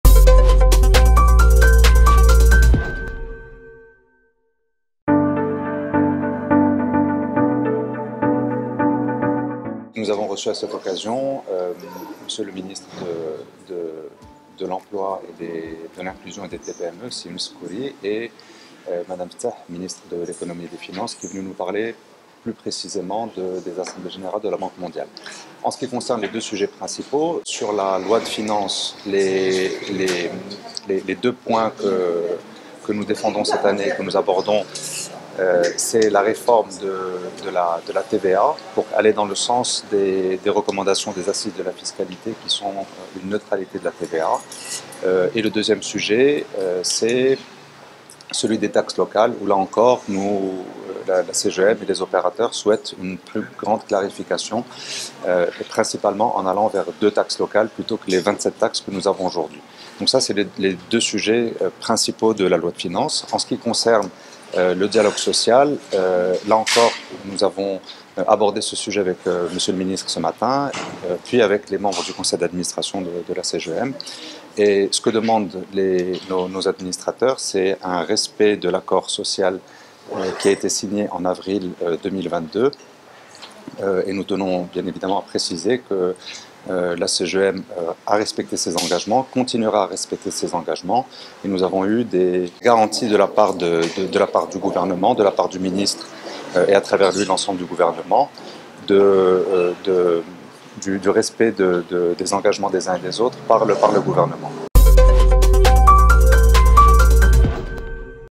الحوار الاجتماعي .. المهدي التازي : حصلنا على ضمانات من الحكومة
قال المهدي التازي، نائب رئيس الاتحاد العام لمقاولات المغرب، إن الاتحاد يتوفر على ضمانات من الحكومة أجل احترام الالتزامات المضمنة في اتفاق أبريل من العام الماضي.
جاء ذلك في تصريح  للصحافة على هامش الندوة الصحفية، التي عقدها الاتحاد العام لمقاولات المغرب، اليوم الجمعة بالدار البيضاء، بهدف الكشف عن انتظاراته من قانون مالية العام المقبل ووضعية الحوار الاجتماعي.
وأعاد التذكير بالرغبة في إعادة تحديد مجال تطبيق الضريبة على القيمة المضافة، وتقليص عدد معدلاتها من خمسة إلى معدلين مع الاحتفاظ بمعدل 0 في المائة، بما يساعد على ضمان حياد هذه الضريبة.
وشدد على ضرورة التوجه نحو رسمين محليين كبيرين، عوض سبعة وعشرين رسما المعمول بها حاليا.
وعند التطرق للحوار الاجتماعي، طالب باحترام الاتفاق الذي وقع مع النقابات في أبريل من العام الماضي، مؤكدا على أن الاتحاد العام لمقاولات المغرب طبق التزاماته، مشددا على أن الاتحاد يتوفر على ضمانات من الحكومة باحترام ما اتفق عليه.

يرجى الاشتراك..